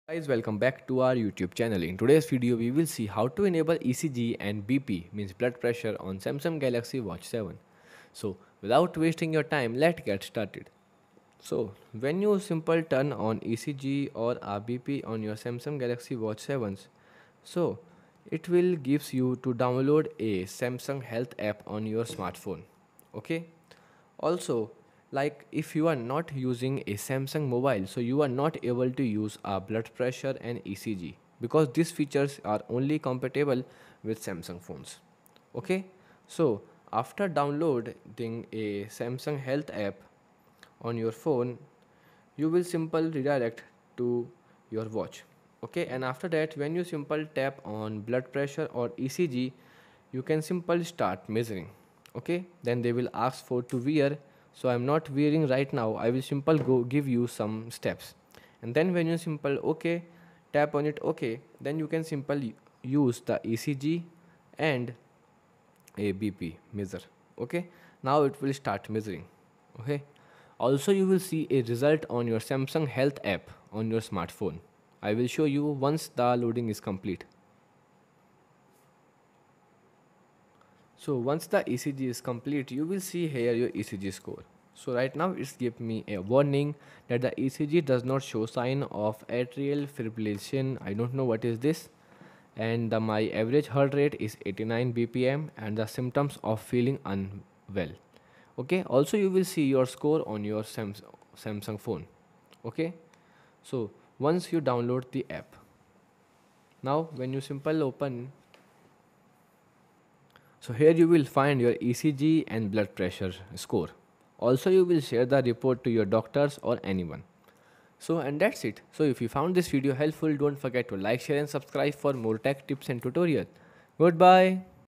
How To Enable ECG And BP On Samsung Galaxy Watch 7
In this video, we walk you through step-by-step instructions on how to ENABLE ECG and Blood Pressure monitoring features. These powerful health tools can help you keep track of your wellness right from your wrist!